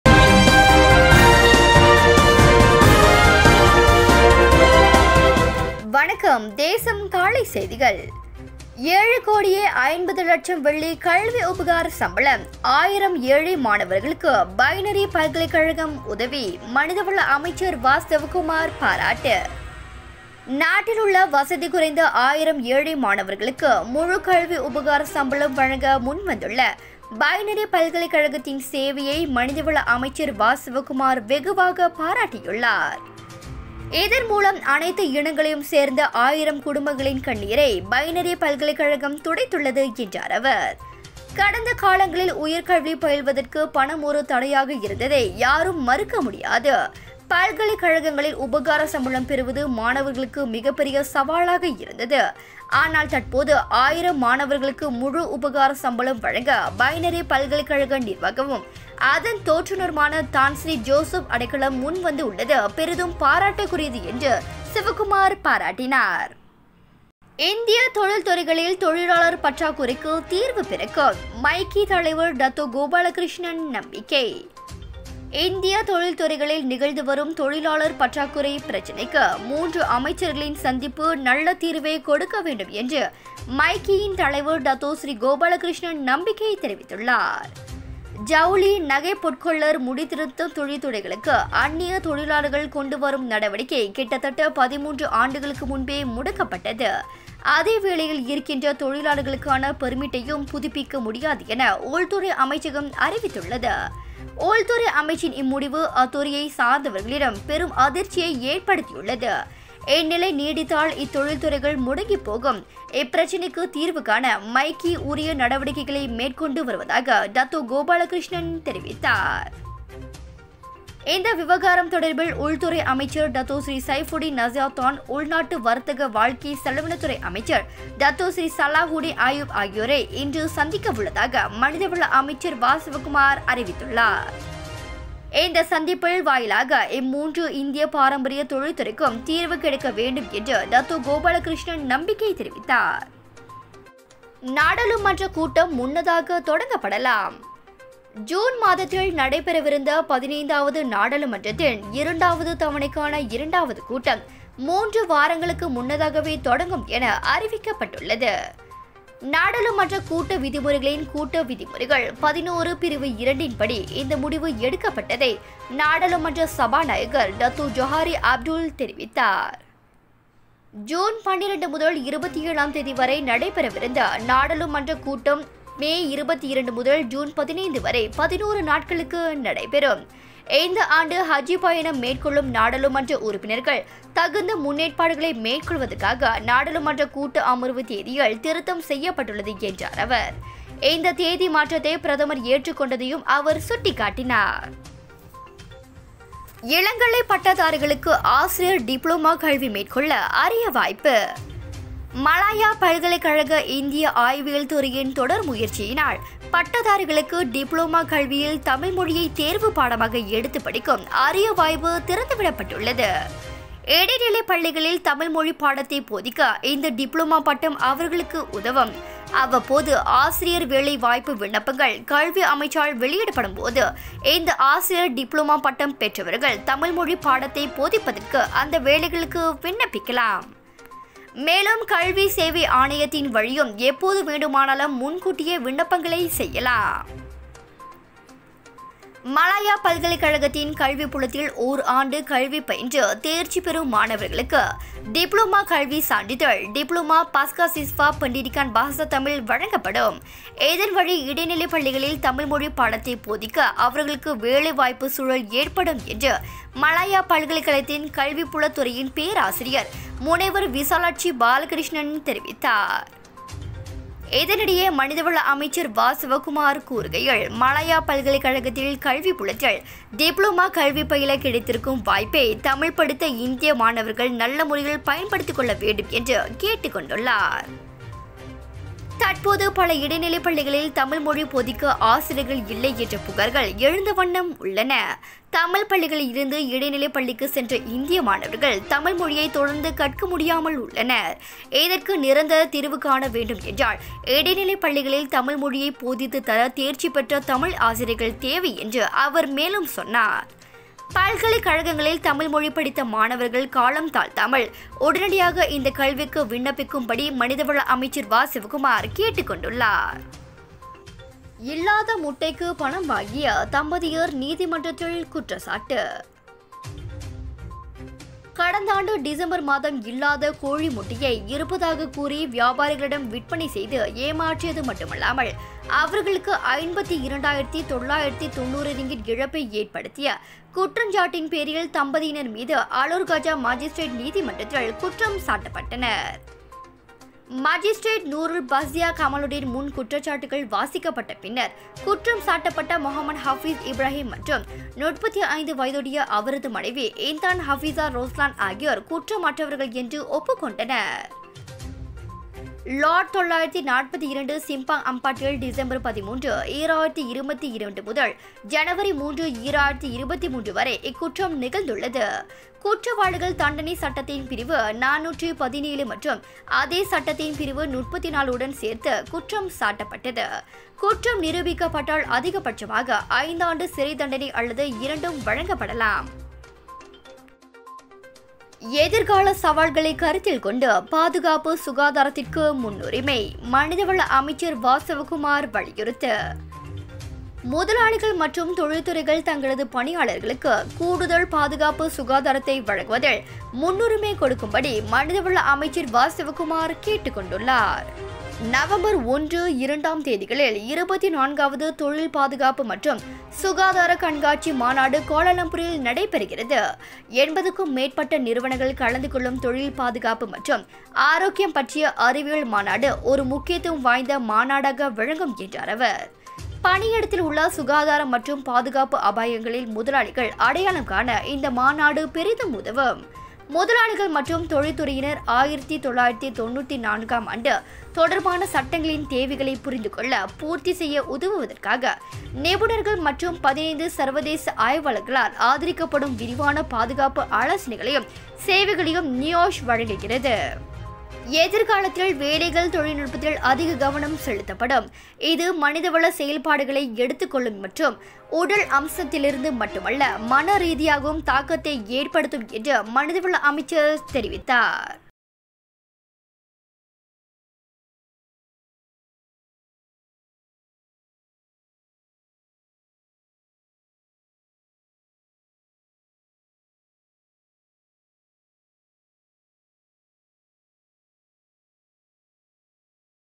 23/3/23 தொழிலாளர் பற்றாக்குறைக்கு தீர்வு பிறக்கும்-டத்தோ கோபாலகிருஷ்ணன் நம்பிக்கை
23/3/23 தொழிலாளர் பற்றாக்குறைக்கு தீர்வு பிறக்கும்-டத்தோ கோபாலகிருஷ்ணன் நம்பிக்கை